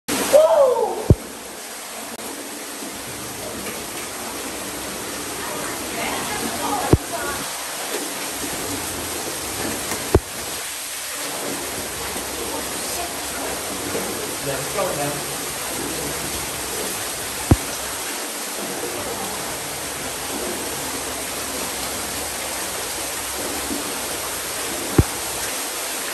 A flooded trip Through Dragon's Tooth Falls
The water was flowing hard today on this trip through the cave at Sims Cave Park in Alabama. These guests had to crawl under Dragon's Tooth Falls.

 *Warning! The actions being performed in this video are being done with guidance from a professional caver in a controlled environment. Never enter a cave without a cave guide.